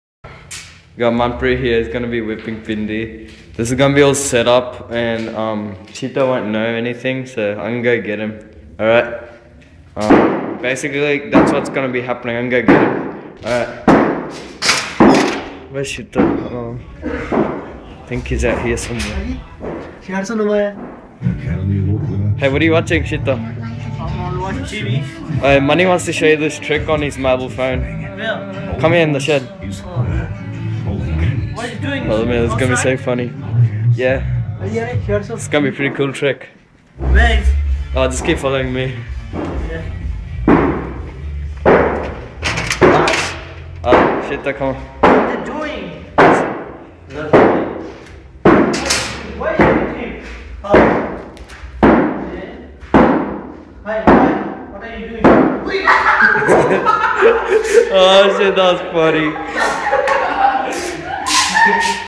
Punjabi boy getting Electric SHOCKED!!
Punjabi boy getting electric shocked by Punjabi girl! FUNNY AS MUST WATCH)%$#@!

!!PLEASE RATE AND COMMENT!!          :D :D :D :D :D  :) :) :)